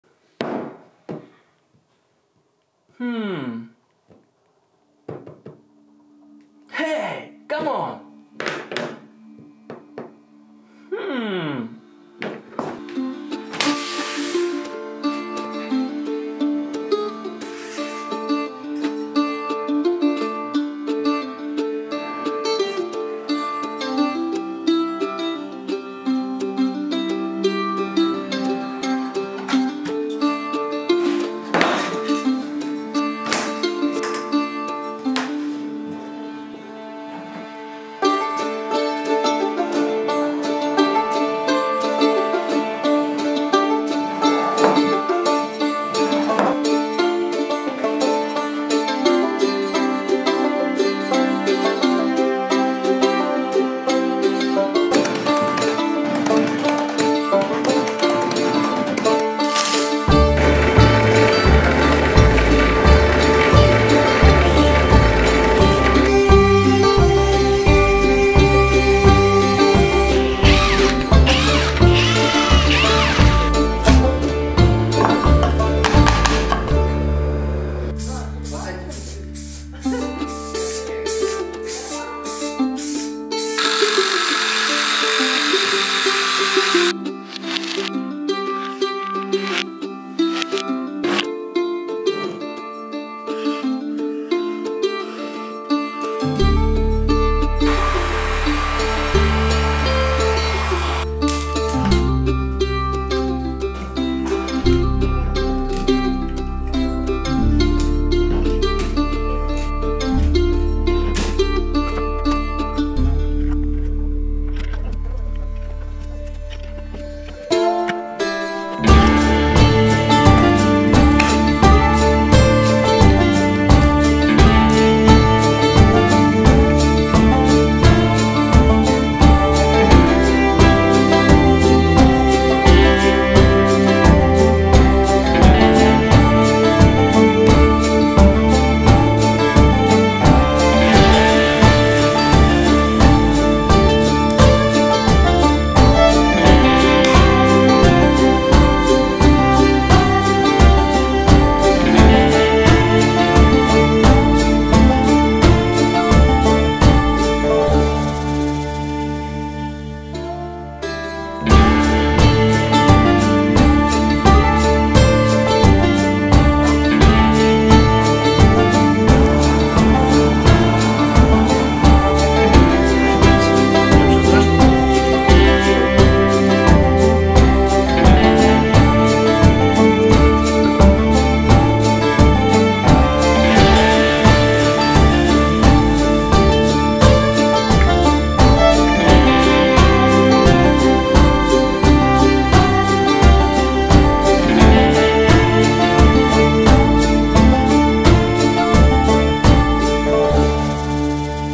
How to make Coca-cola Charger
How sometimes there is not enough iPhone charge, and only Coca Cola is at hand. Well, let's try to get out of the tricky situation when we have a bottle of Coca-cola and a discharged phone.